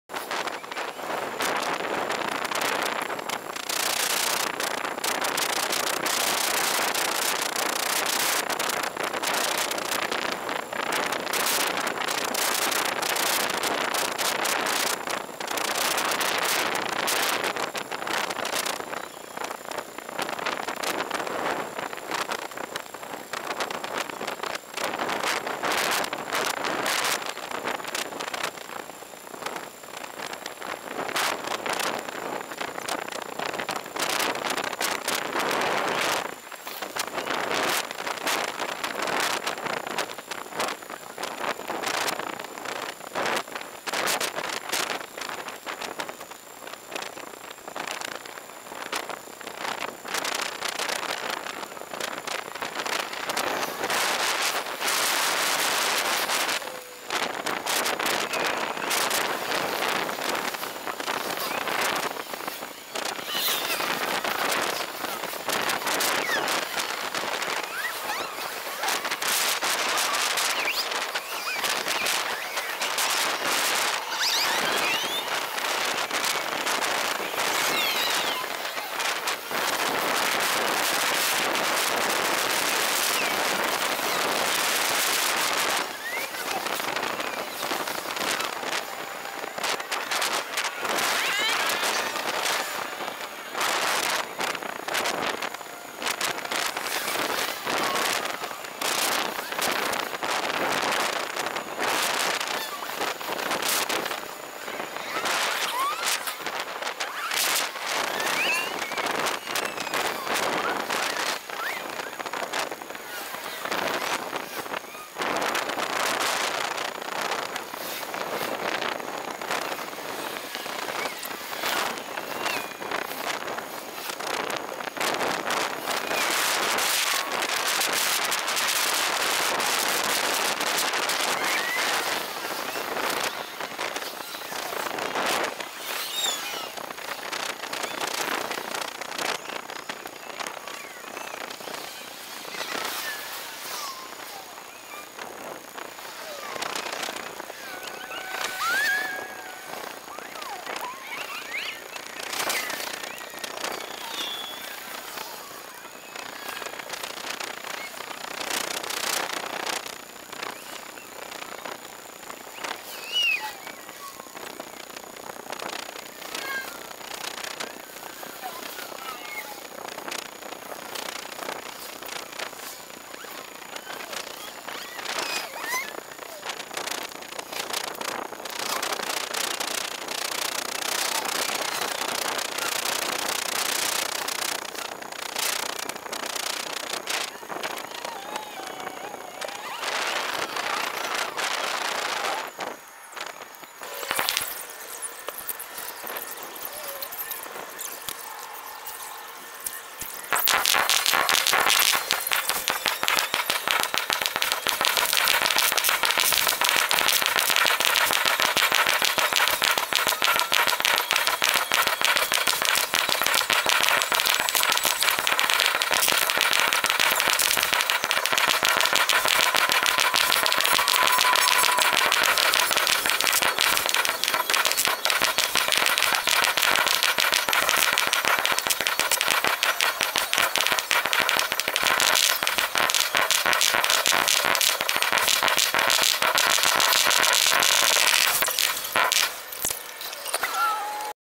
Fireworks run
Ran the 18 miles to & from the fireworks.  Only had enough battery power to record 6 miles.  Forgot this location was directly under the fireworks & didn't aim high enough.  It's a glimpse into how the rest of the world is going to make handheld, long exposures & timelapses with the 3 axis brushless gimbal.